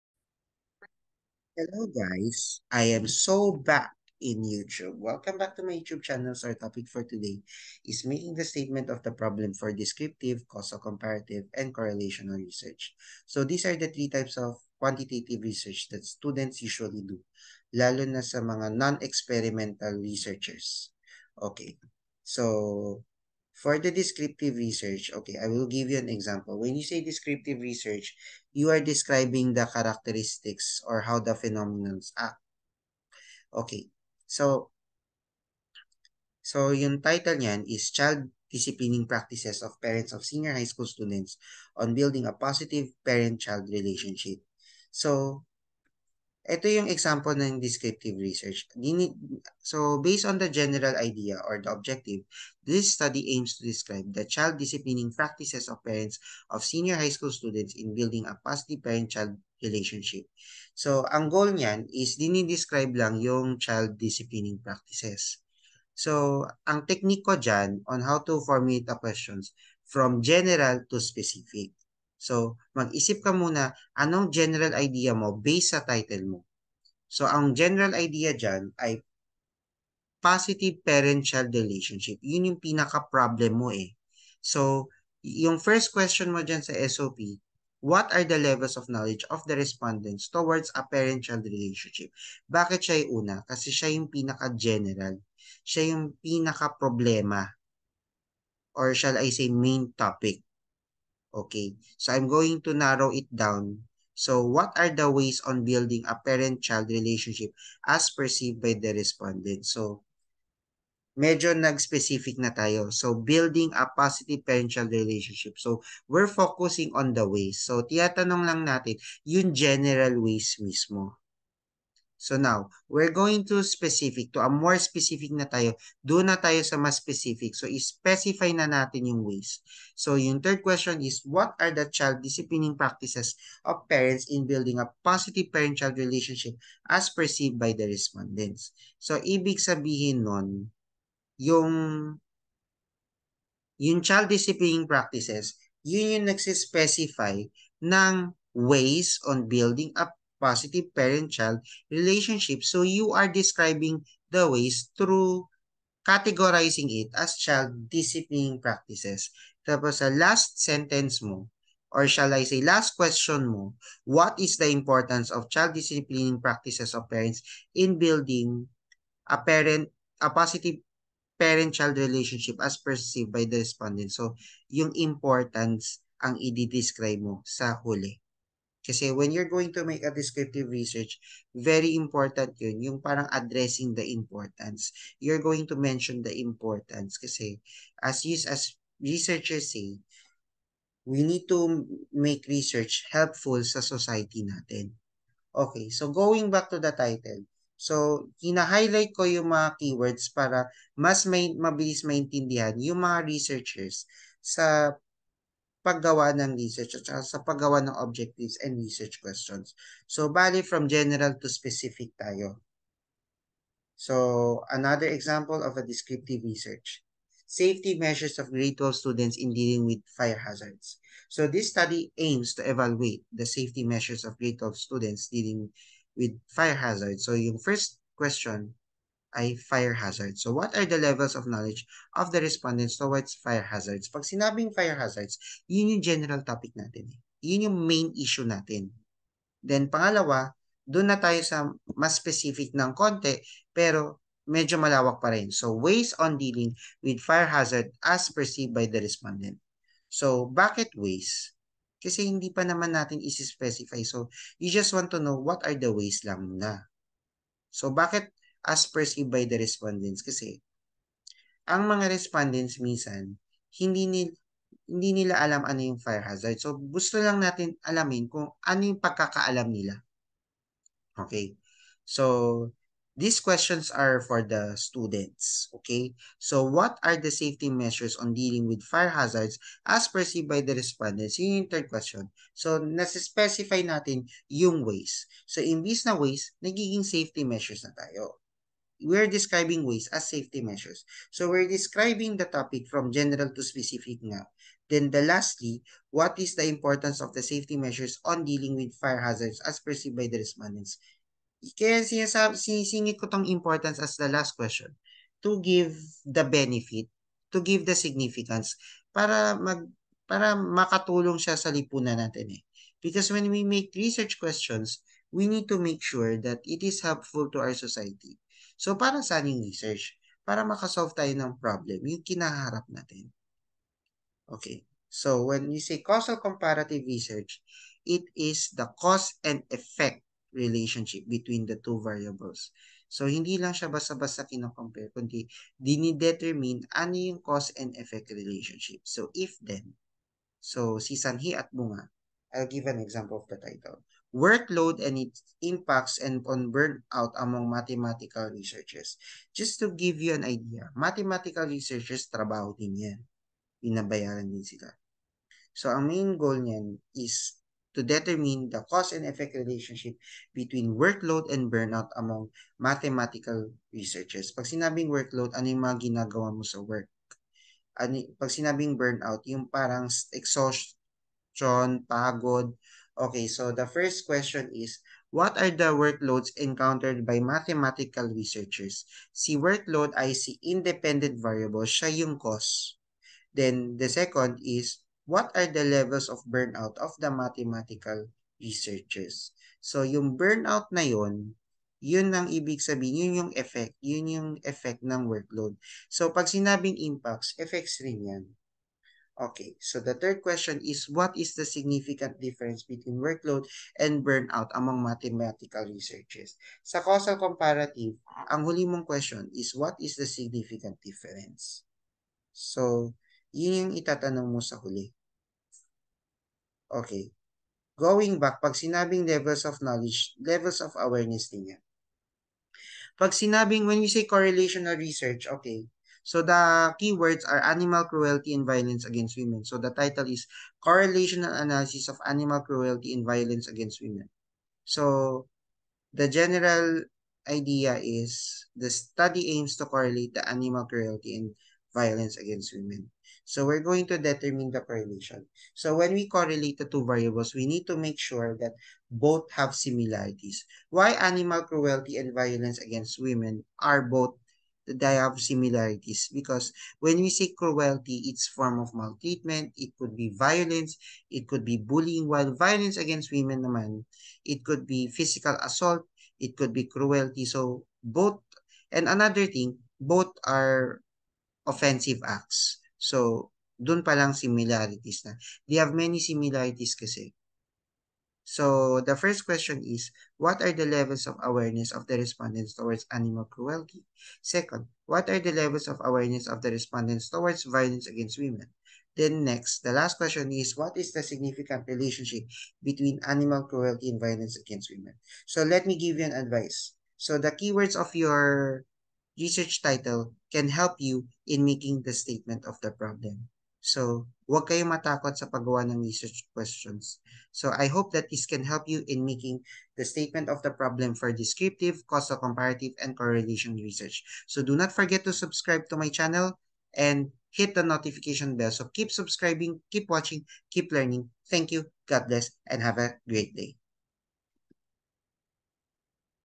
Making a Statement of the Problem for Descriptive, Causal Comparative, and Correlational Research
Hello... Here are the tips when you make a Statement of the Problem for these research:
1. For Descriptive, consider including the importance but better make questions from general to specific.
2. For Causal-Comparative, always include a question that is about significant difference. But in addendum, you can also include a question that is about significant relationship in order to test the relationship.
3. For Correlational, always include a question that is about significant relationship.
NOTE: Use keywords of your title so you can have an idea in making a Statement of the Problem.